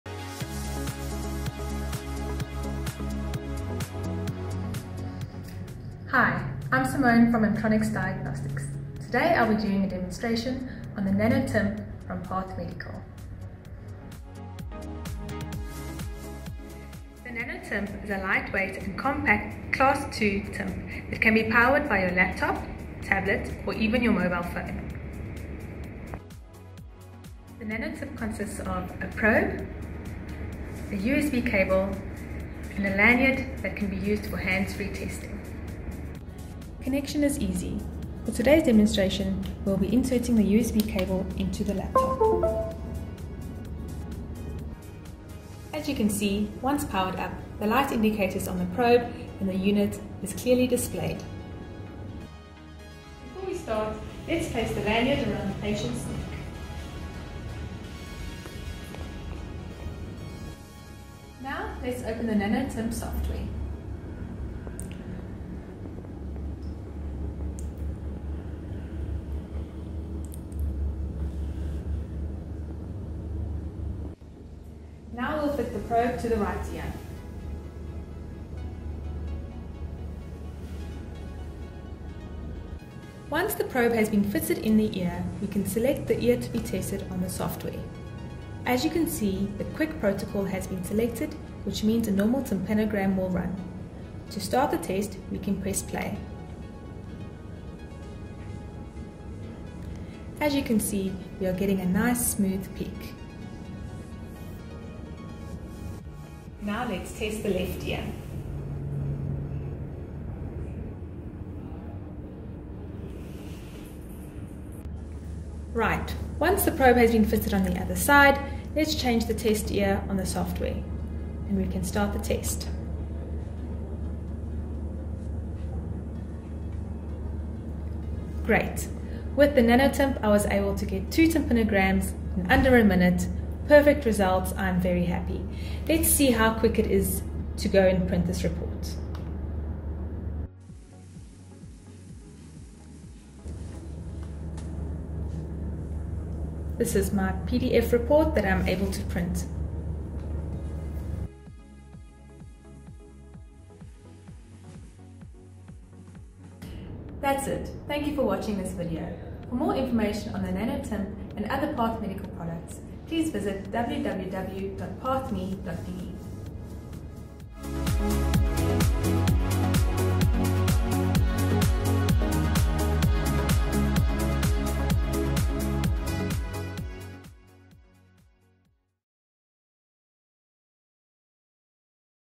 PATH MEDICAL nanoTymp demonstration
This video demonstrates the new nanoTymp unit from PATH MEDICAL. It's the worlds first PC based Tympanometer that can be connected to PC, tablet or even your mobile phone!